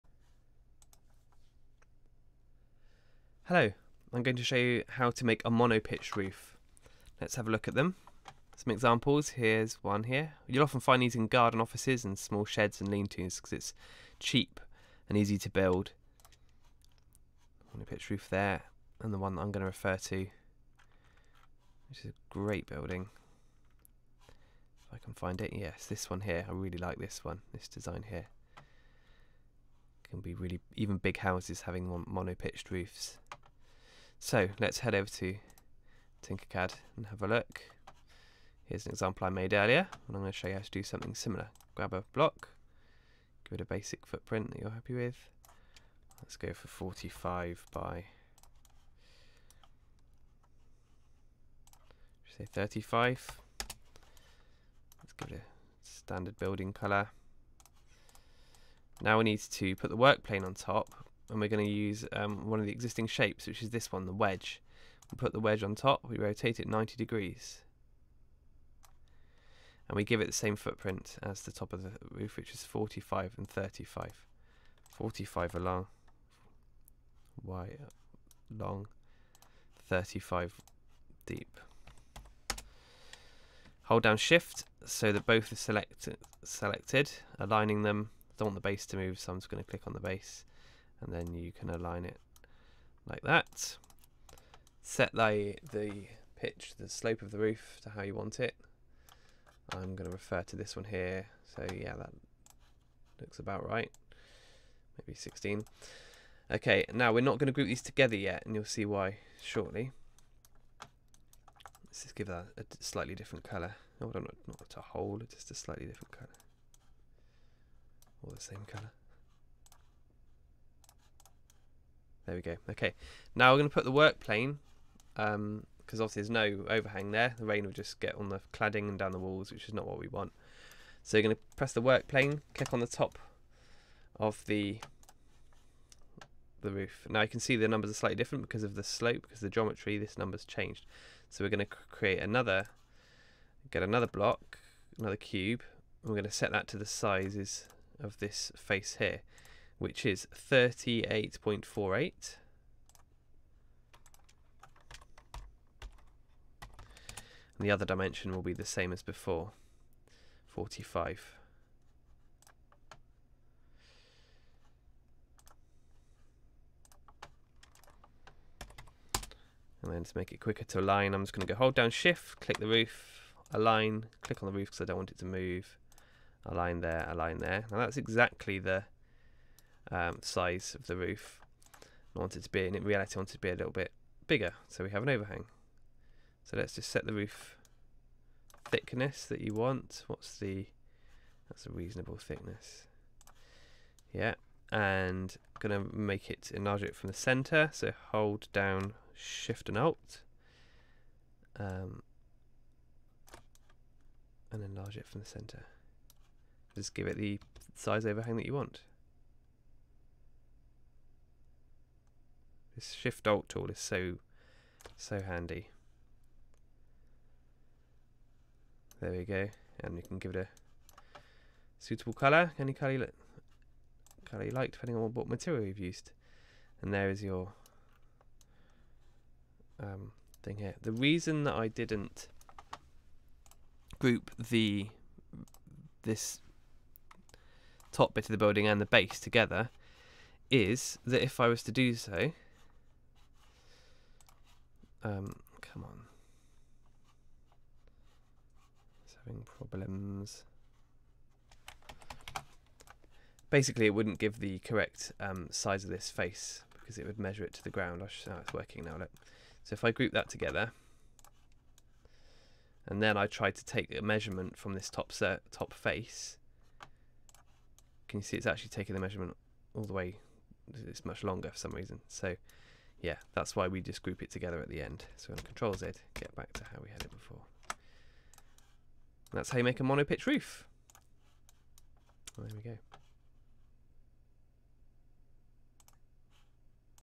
Mono Pitched Roof Tutorial
How to make a mono pitched roof in Tinkercad